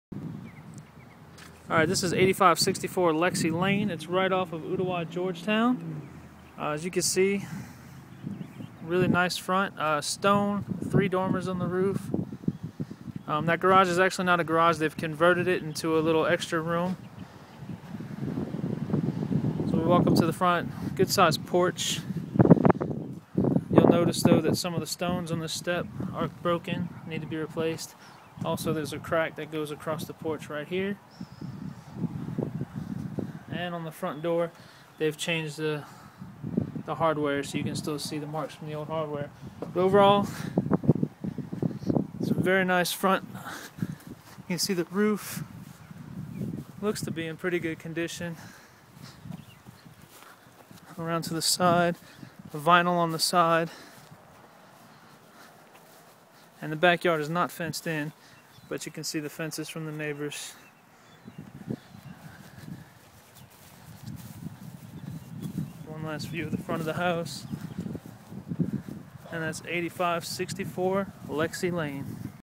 8564 Lexie Ln exterior video.MOV
8564 Lexie Ln is a HUD owned home for sale in Ooltewah TN